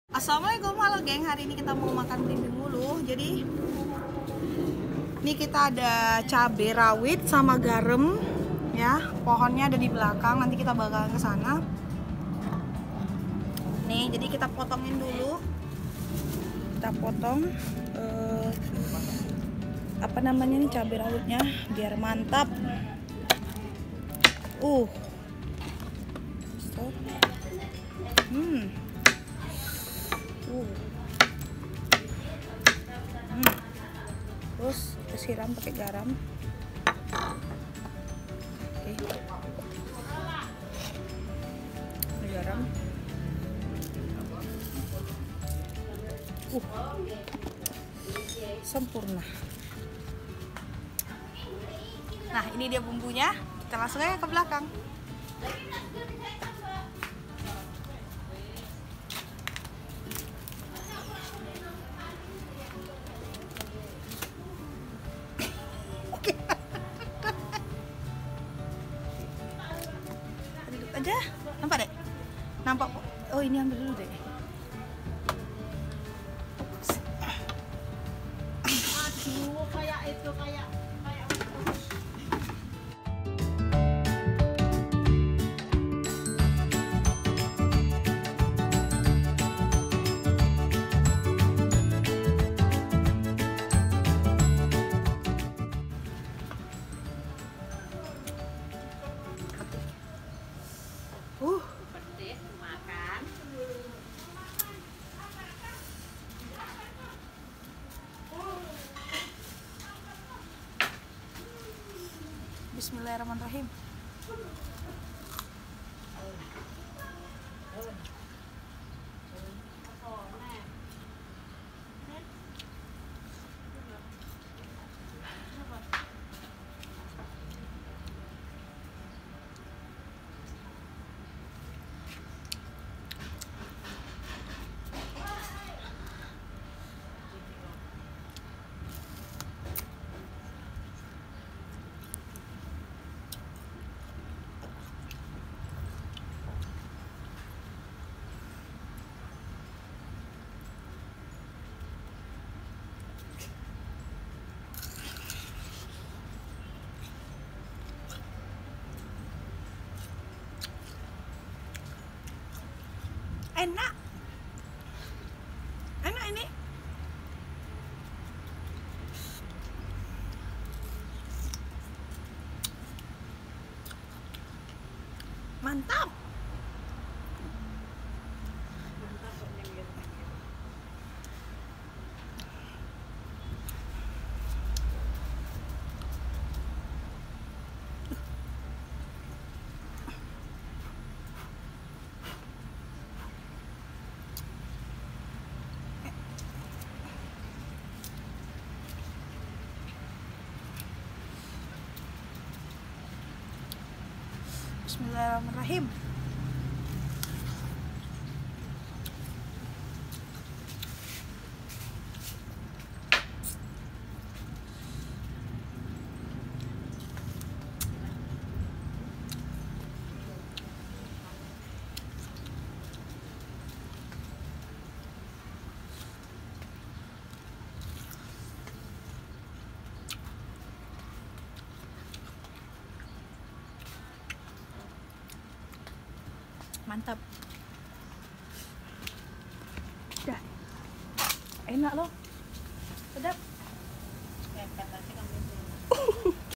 Belimbing Wuluh Kasi Cabe & Garam Bikin muka Gak Kontrol
Video by : iphone 7+
lokasi di Rumah Makde imam,Perumahan Greenland batam.

rasa makan nya,kecut kecut seger tapi gak mau berenti 😂😂